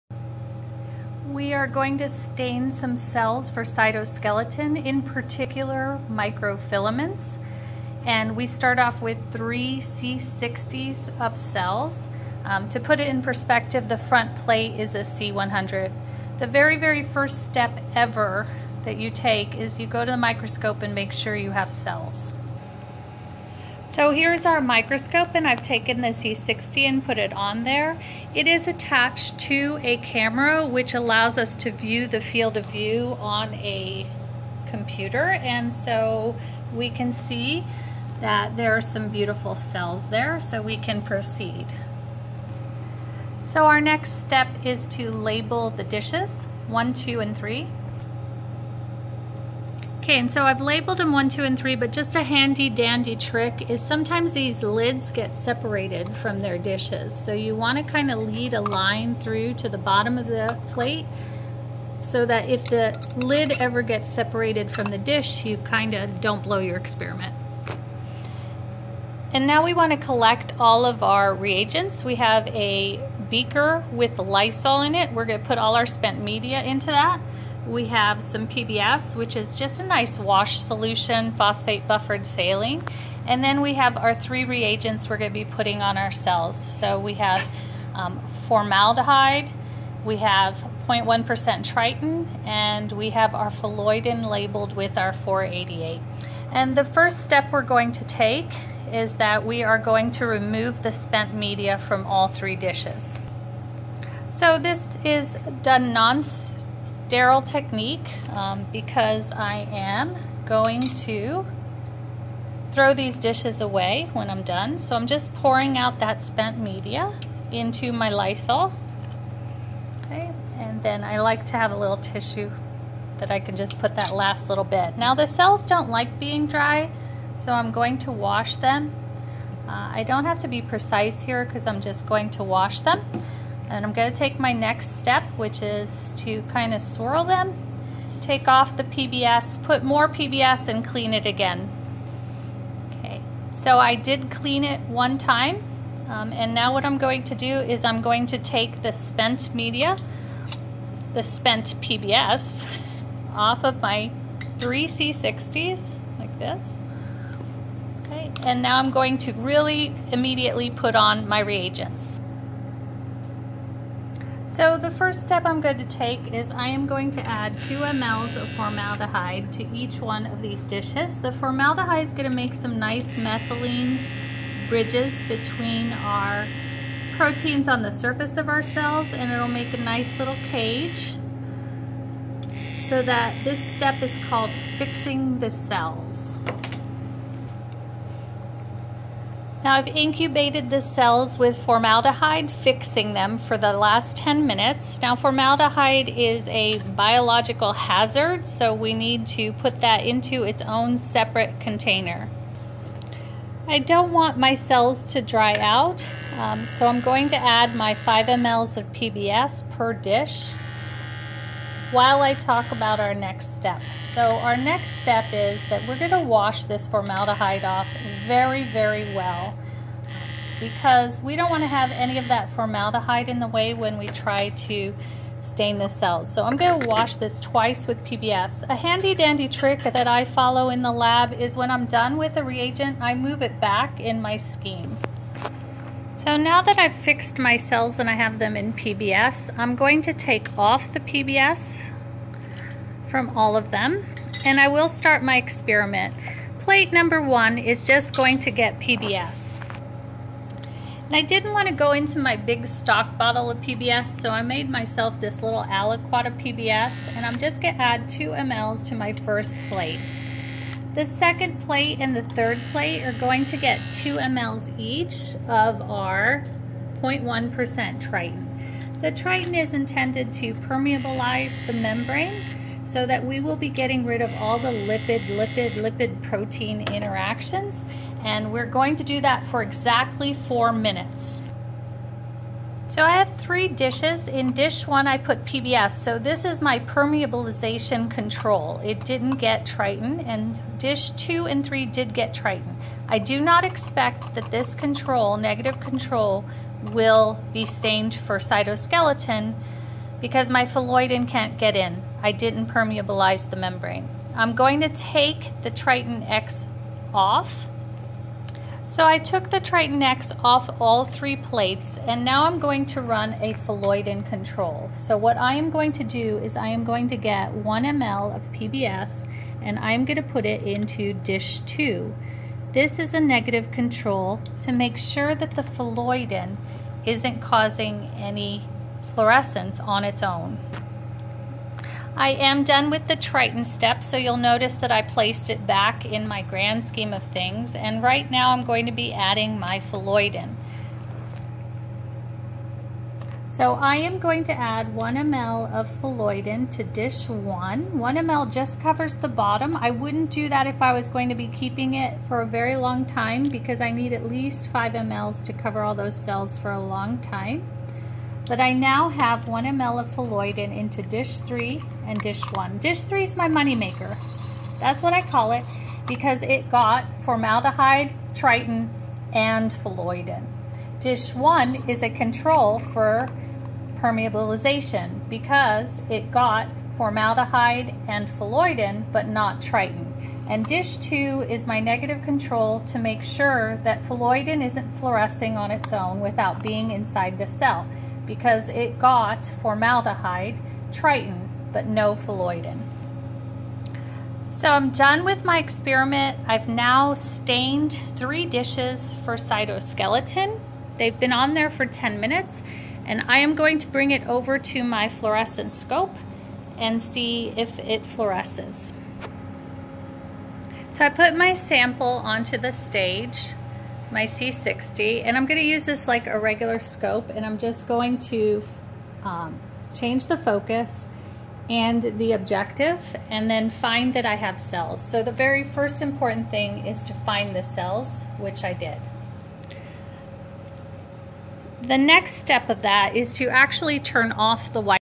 Staining Actin Filaments
GV Cell Biology Laboratory Demo on how to stain microfilament (aka actin filaments) with Phalloidin